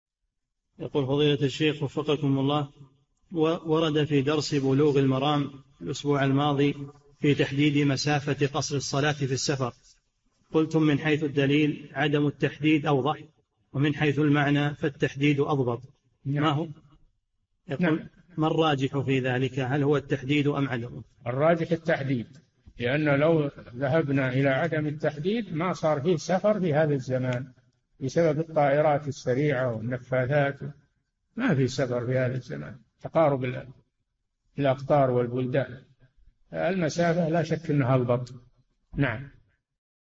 [3219- 4717] هل الراجح تحديد مسافة السفر أم عدمه ؟ - الشيخ صالح الفوزان
"[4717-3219] ورد في درس [بلوغ المرام] في الأسبوع الماضي في تحديد مسافة قصر الصلاة في السفر قلتم: من حيث الدليل عدم التحديد أوضح ومن حيث المعنى فالتحديد أضبط، ما الراجح في ذلك: هل هو التحديد أم عدمه؟
الجَوَابُ: الراجح التحديد؛ لأننا لو ذهبنا إلى عدم التحديد ما صار في سفر في هذا الزمان بسبب الطائرات السريعة والنفاثات ما في سفر في هذا الزمان لتقارب الأقطار والبلدان، فالمسافة لا شك أنها أضبط.

للاشتراك في قناة الشيخ صالح الفوزان
فتاوى المجالس في تفسير المفصل

للاستفسار والإقتراحات والدعم
رئيس الفريق
لمشروع كبار العلماء